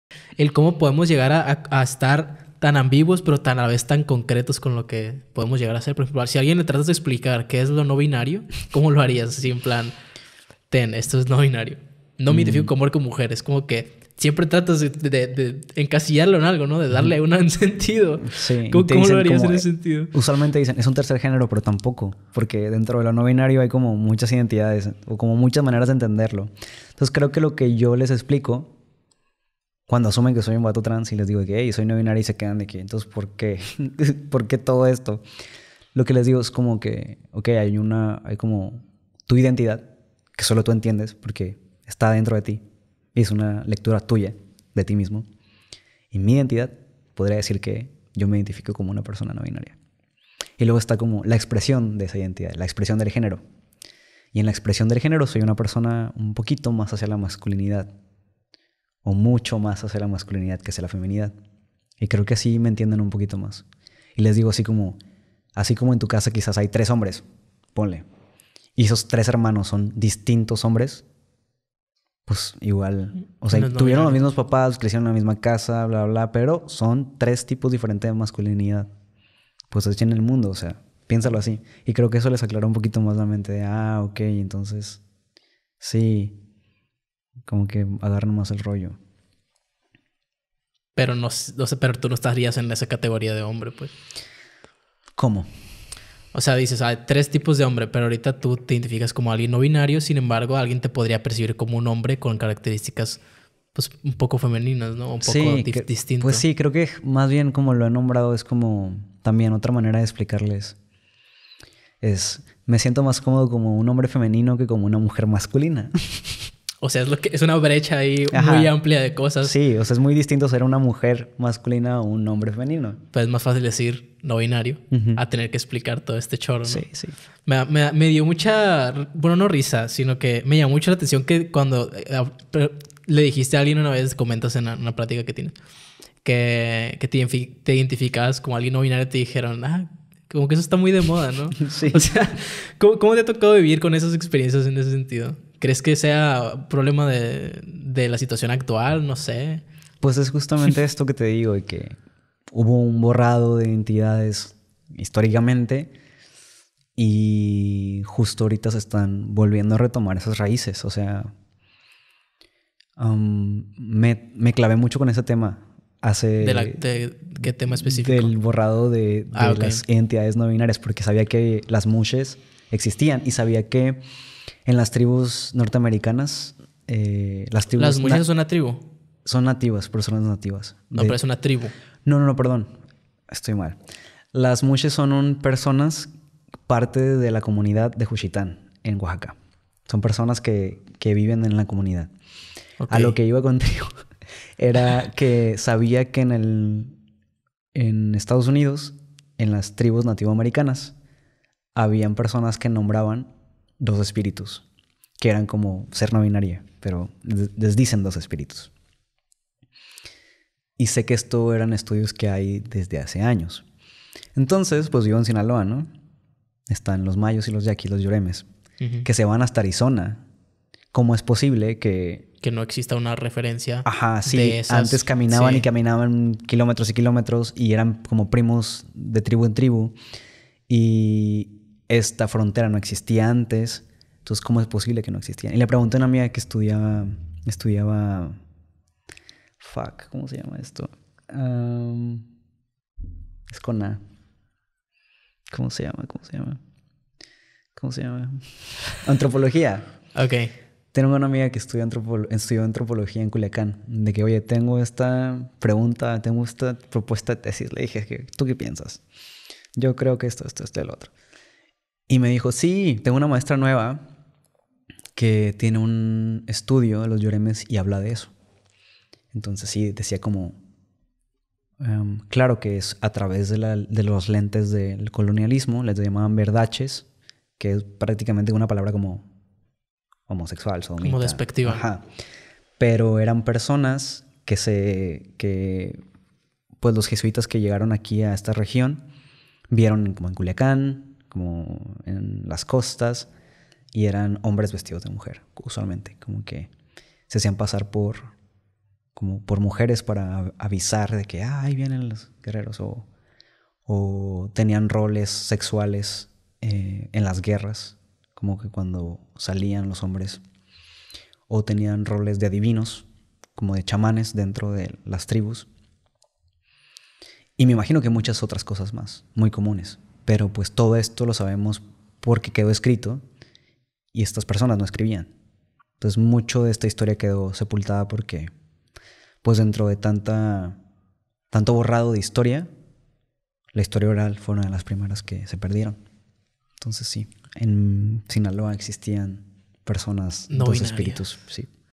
SER NO BINARIO CON DANTE URETA II ESTE PODCAST YA EXISTE (O NO) II
Cómo describir la ambiguedad de lo no binario.